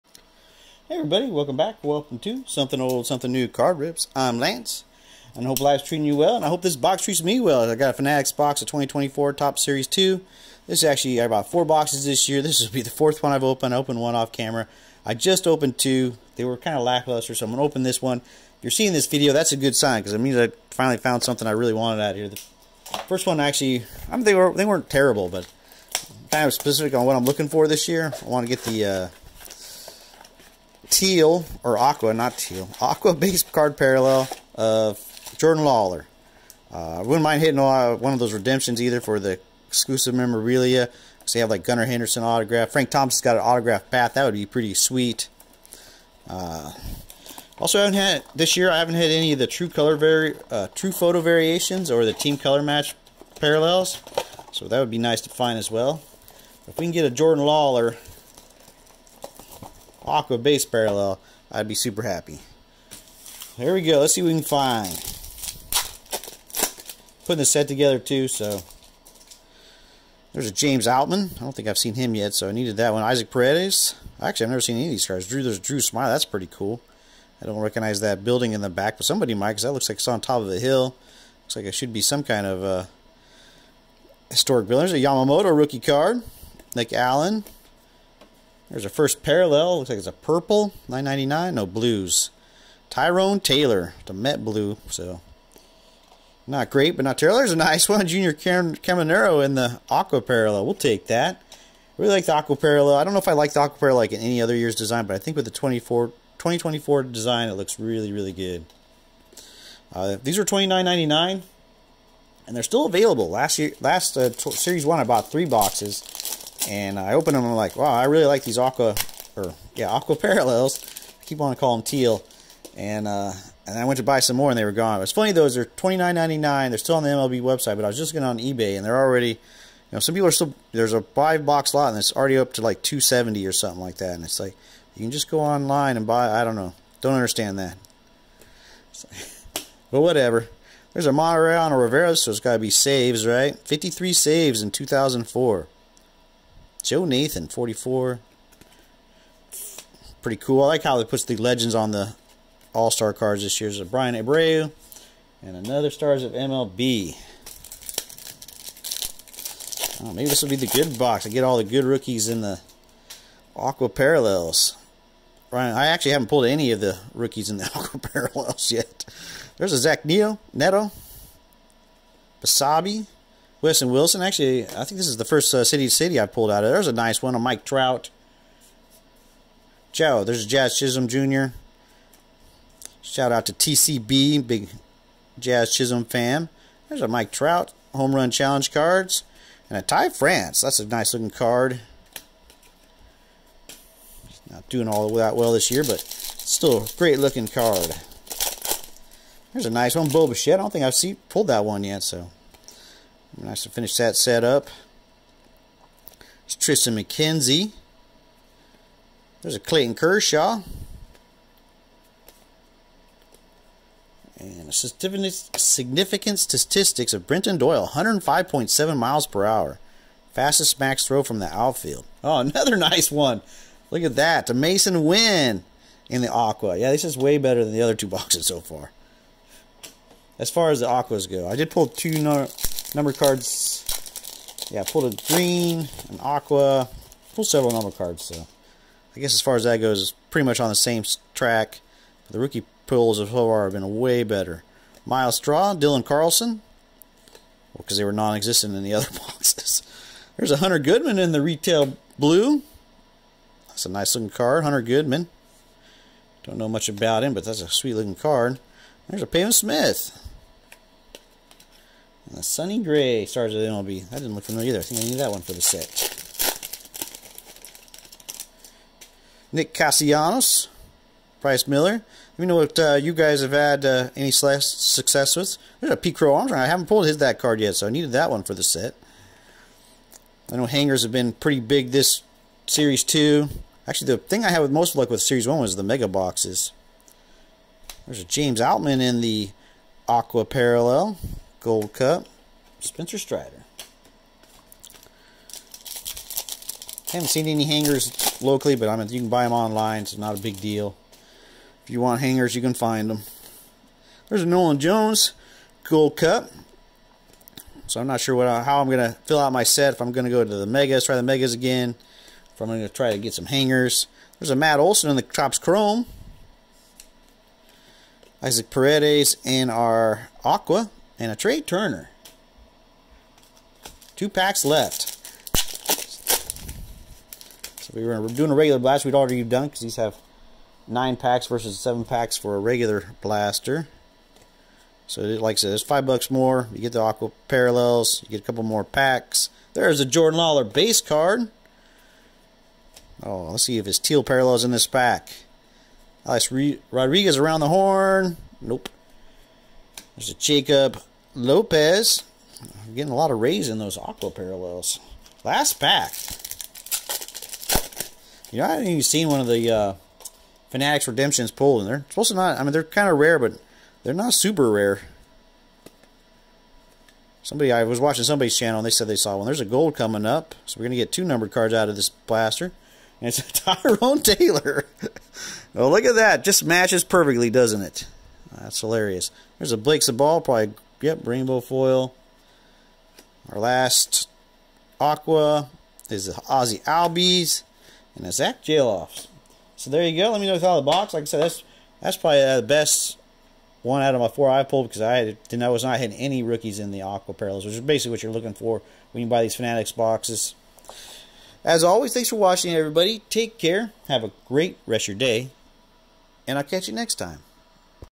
2024 Topps Series 2 Fanatics Blaster
Welcome everyone to another baseball card blaster box rip. Today we have a value box of the Fanatics exclusive 2024 Topps Series 2. It's all about the teal (I mean aqua 😉) parallels and I finally found a few good ones in the fourth box I ripped into. What do you think of this format? Have you had any luck with it or plan on trying it?